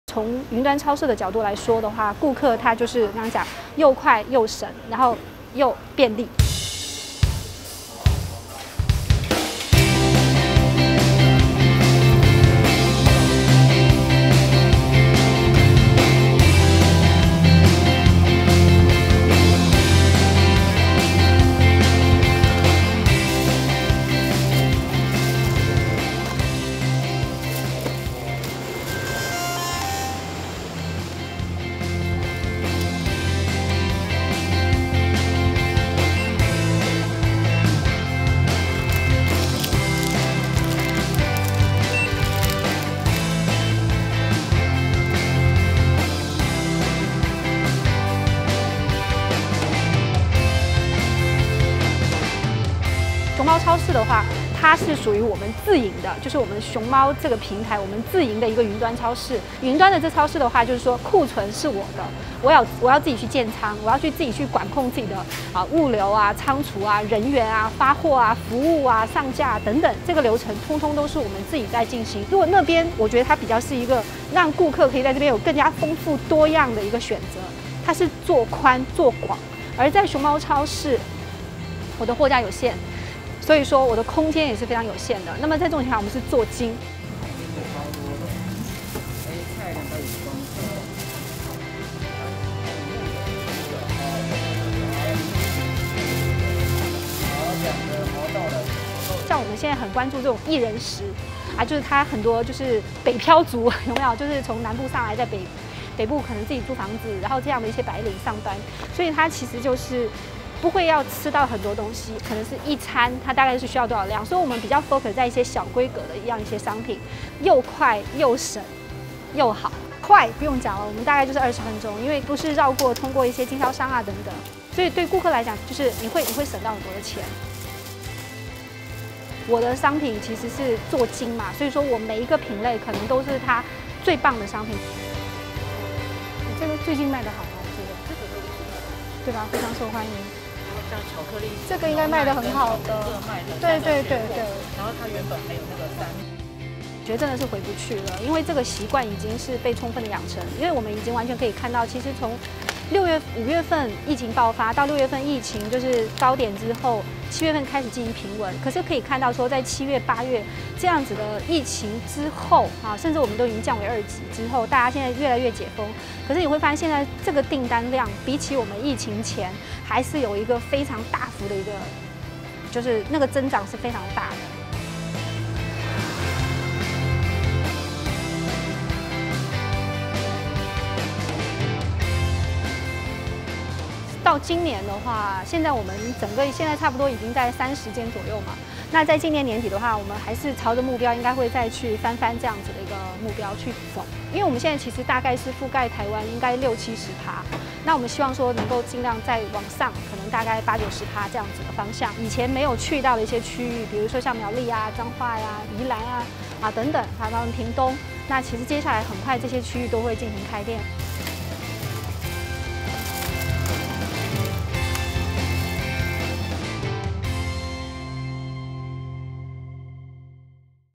外送平台搶進自營超市 foodpanda在想什麼？
外送平台紛紛搶進自營超市，foodpanda不再只是外送餐點，砸下重資開設雲端超市。總監何禮旭表示，熊貓超市從建倉、庫存及物流一手包辦，且主打「一人食」小規格商品，搶攻白領族市場。疫情改變消費習慣，即便國內防疫降級，超市訂單量仍居高不下，也讓foodpanda信心滿滿，認為雲端超市的外送商機無限。